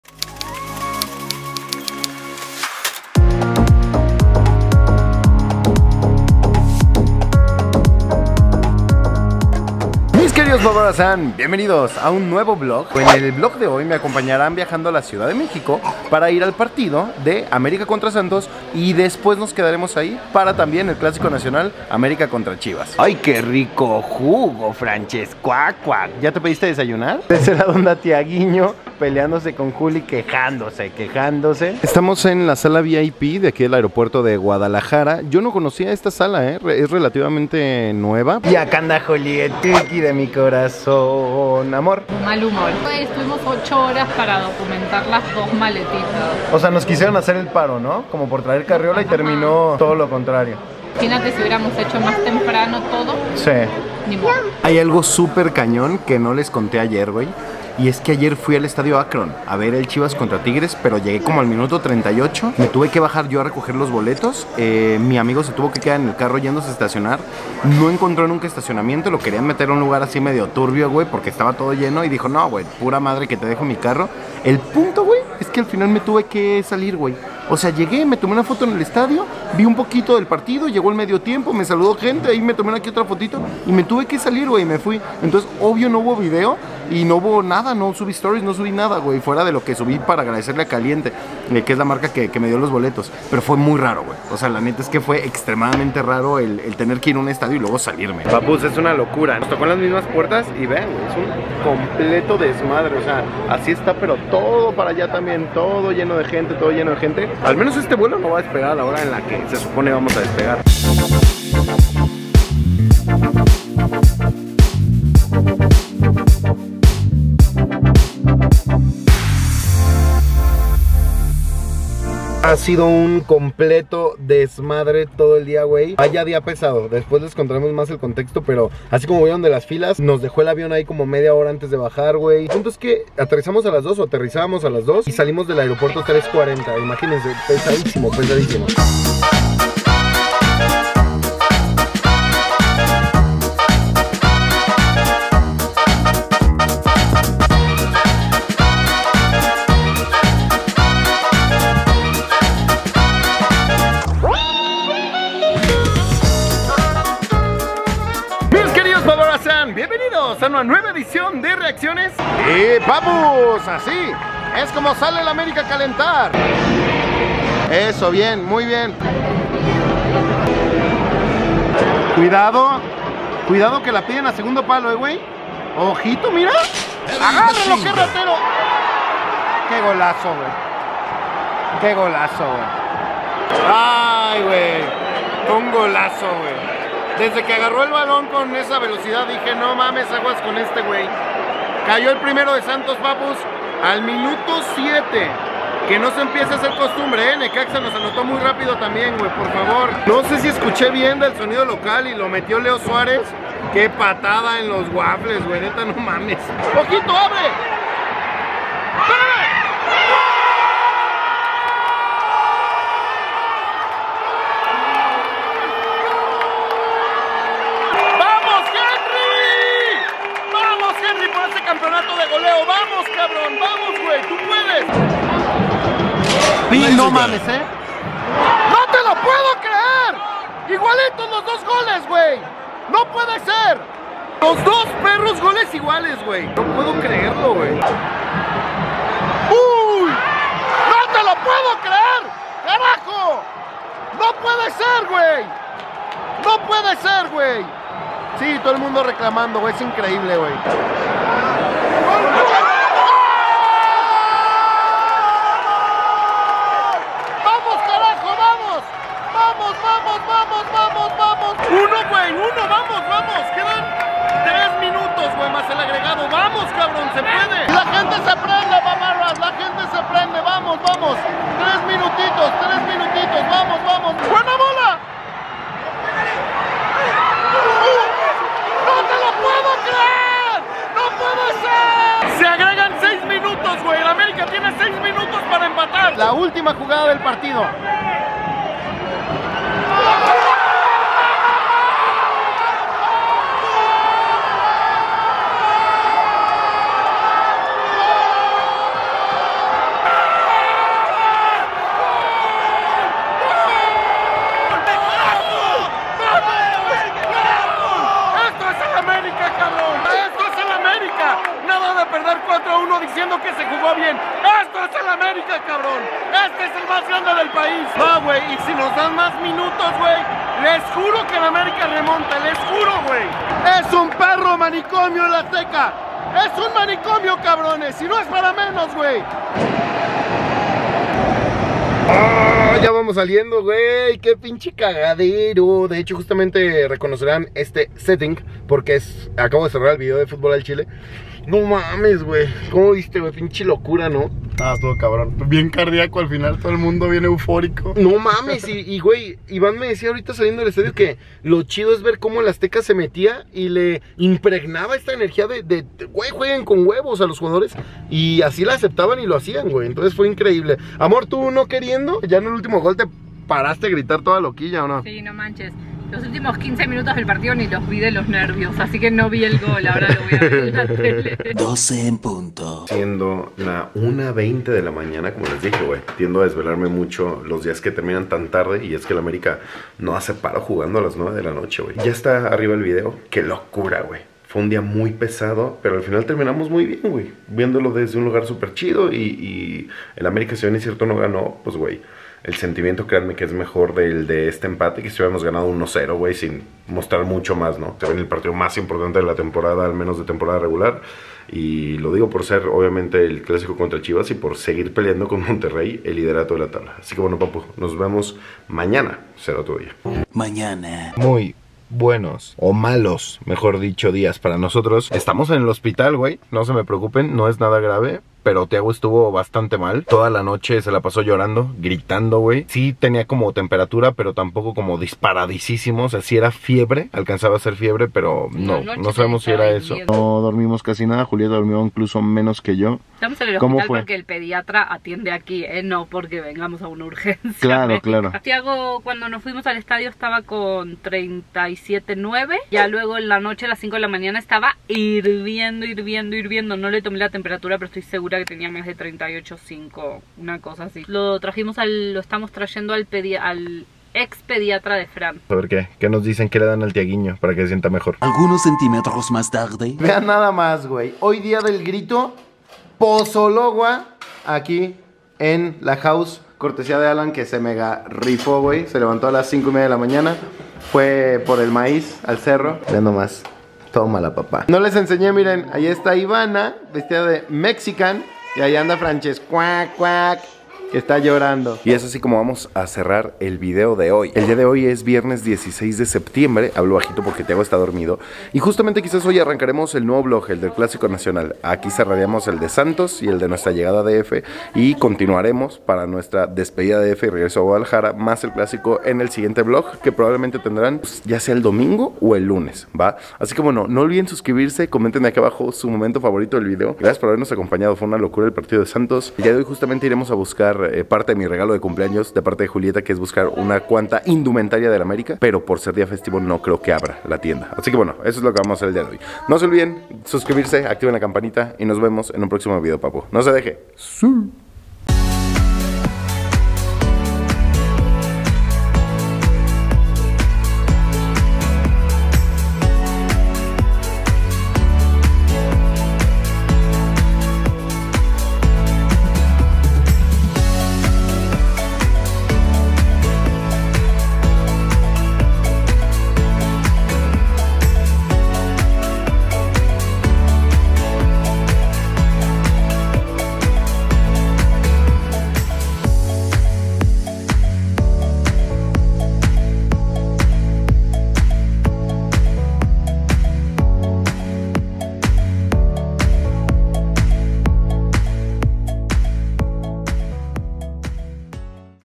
¡PARTIDAZO ÉPICO EN EL AZTECA! Vlog América vs Santos
América empató a Santos con gol de Viñas a último minuto y enfrentará a Chivas mañana en clásico nacional América vs Chivas, no se pierdan las reacciones en Futbol al chile.

Subs: 45,984